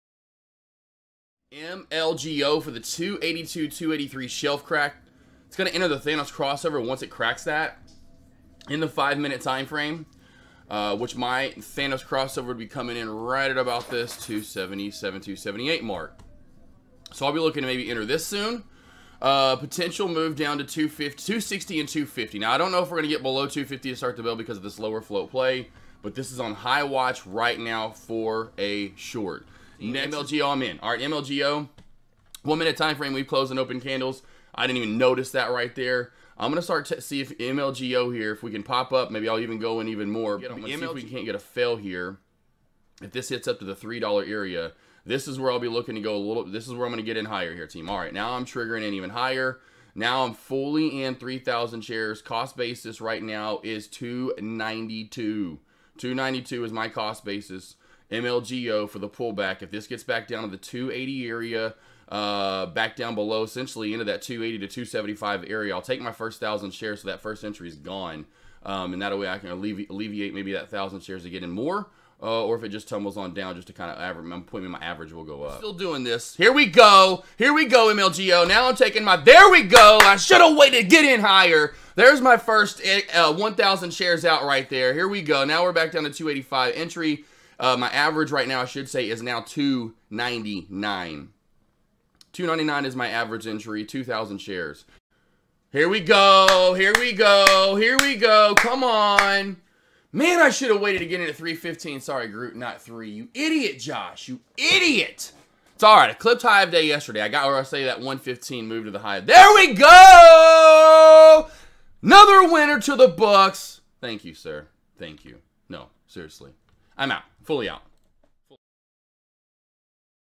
$MLGO LIVE Short Trade - Live stock trading at Tradecaster.com
$MLGO LIVE short trade today - Took the opening bell short opp once this hit RES at PM highs of $3 area, for a fail back to $2.65. Take a watch at how to scale into and out of a short.